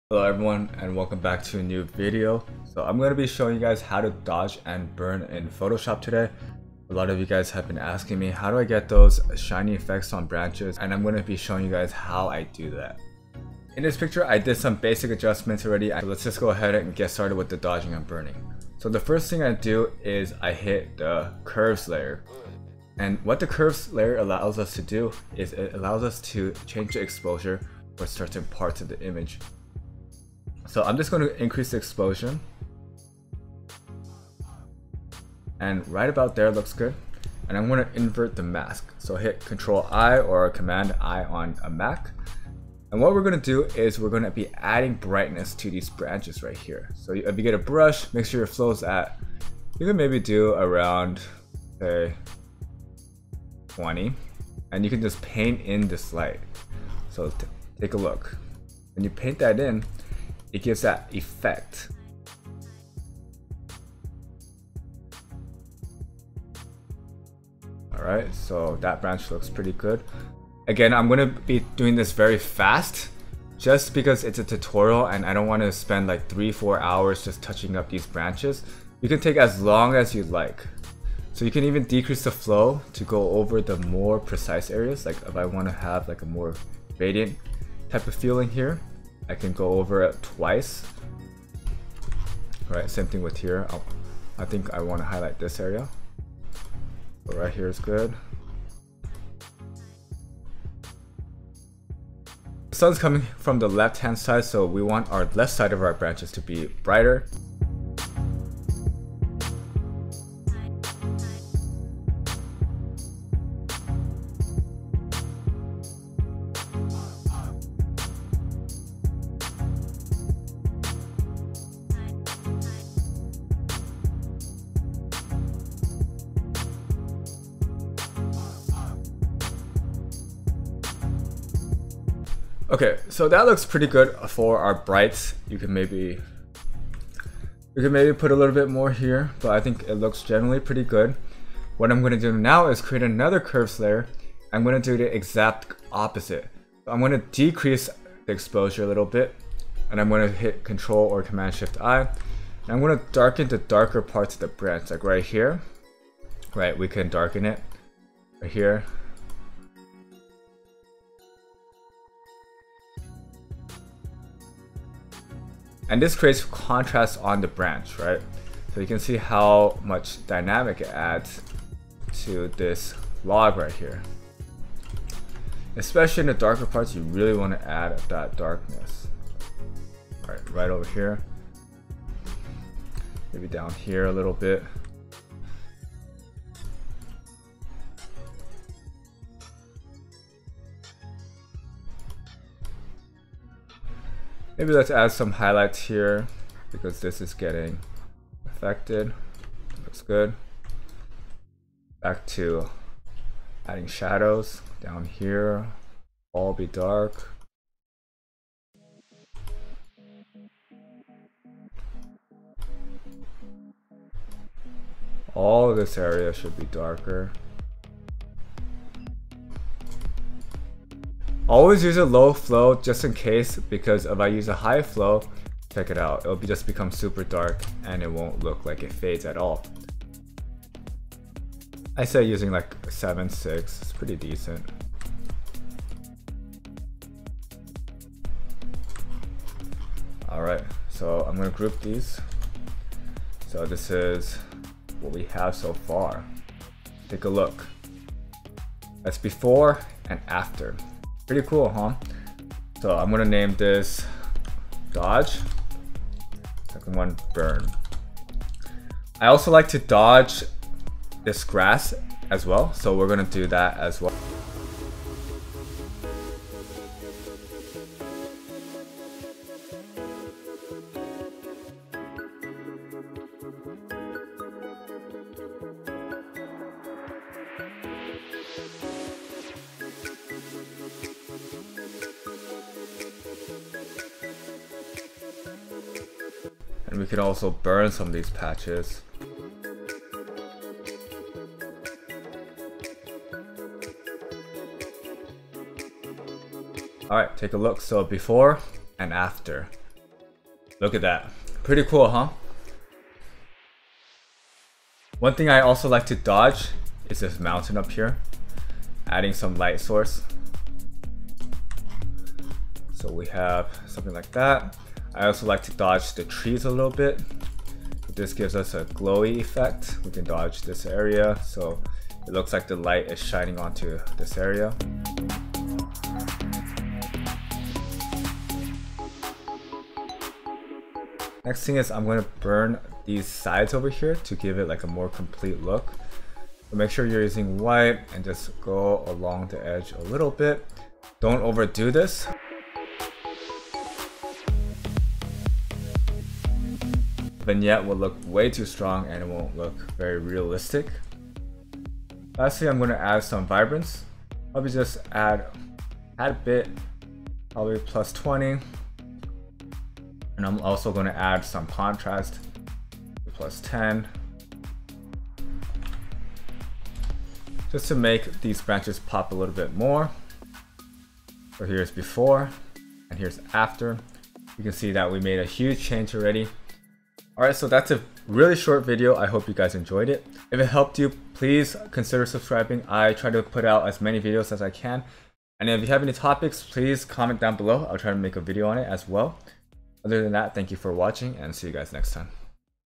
How to Dodge and Burn in Photoshop
I received a few questions regarding on how to make the branches glow/shine/etc. and the solution is to dodge and burn. This is a technique I use in many of my photos to create more dynamic contrast.